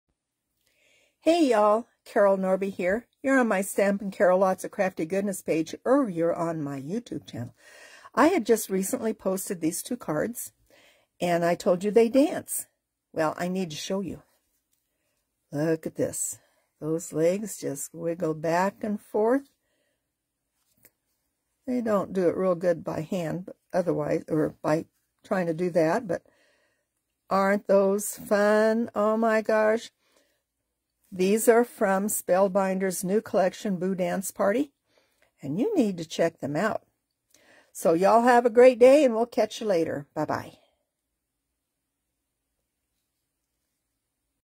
Dancing Cards
These cards were made using 2 new die sets from Spellbinders new Boo Dance Party collection. The legs swing back and forth!